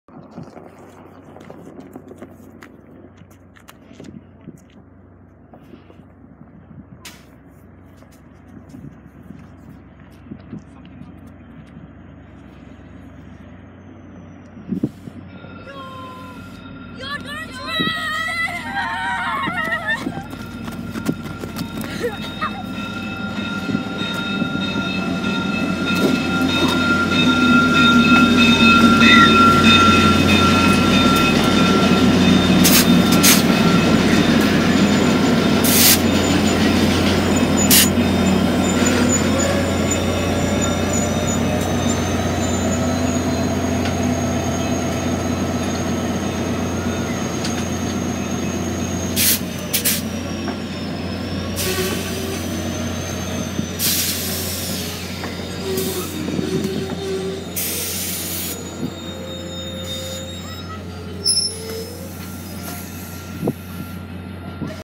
VIA Rail 693 Arriving Ilford 04/17/23
VIA Churchill train arriving at 10:10 pm in Ilford